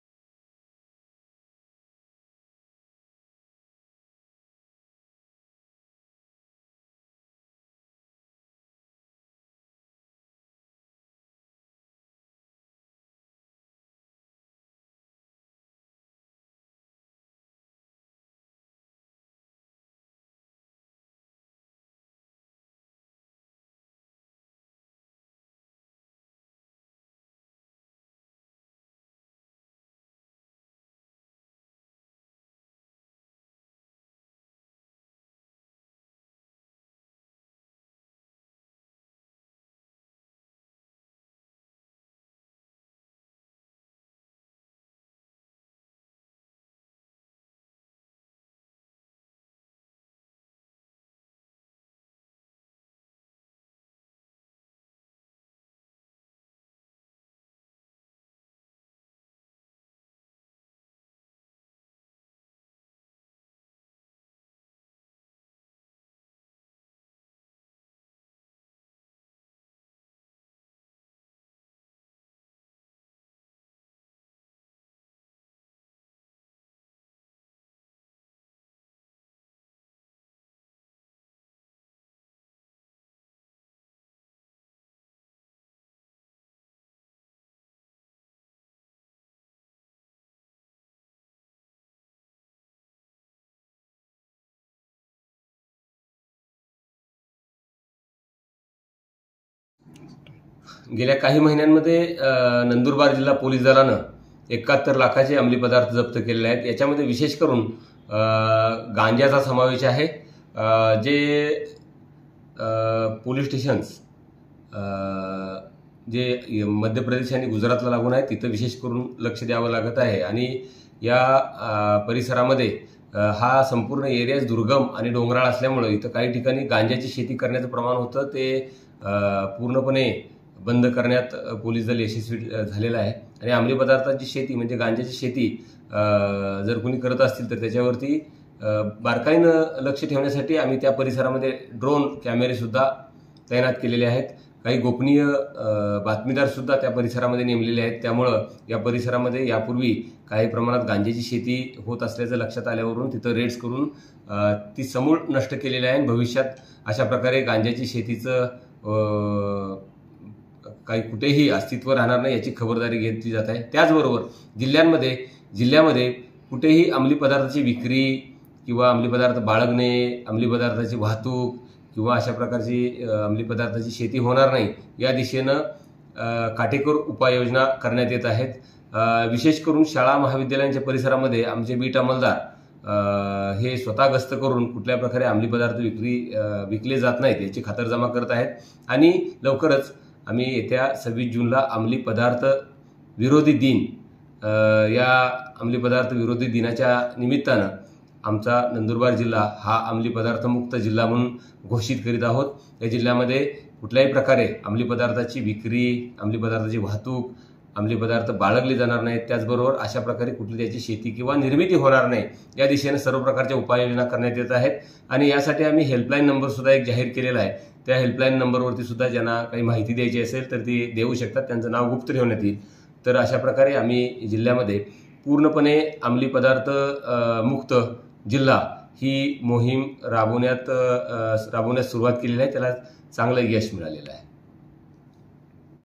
NANDURBAR | अमली पदार्थांवर आळा घालण्यासाठी जिल्हा पोलीस दलाची विशेष मोहीम
नंदुरबार जिल्हा हा सातपुड्याच्या डोंगररांगांमध्ये आणि गुजरात, मध्यप्रदेश राज्याचा सीमा वरती भागात आहे. त्यामुळे या भागात काही ठिकाणी चोरून गांजाची शेती केली जात असते, या गोष्टी लक्षात घेऊन नंदुरबार जिल्हा पोलीस दलाच्या वतीने दुर्गम भागावर लक्ष ठेवण्यासाठी ड्रोन द्वारा तर सपाटी भागात शाळा महाविद्यालय परिसरात बीट मार्शल यांच्या माध्यमातून अमली पदार्थांना आळा घालण्यासाठी विशेष मोहीम सुरू केल्या असून, पोलिसांना यश येत असल्याचे चित्र आहे. येत्या 26 जून रोजी अमली पदार्थ विरोधी दिन असून या दिवशी जिल्हा अमली पदार्थ मुक्त घोषित केला जाणार आहे. अशी माहिती पोलीस दलाच्या वतीने देण्यात आली आहे. नंदुरबार जिल्हा पोलीस दलाच्या वतीने गेल्या सहा महिन्यात विविध ठिकाणी धाडी टाकून ७२ लाखाचे अमली पदार्थ जप्त केले आहेत.

BYTE :  पी आर पाटील - पोलीस अधीक्षक नंदुरबार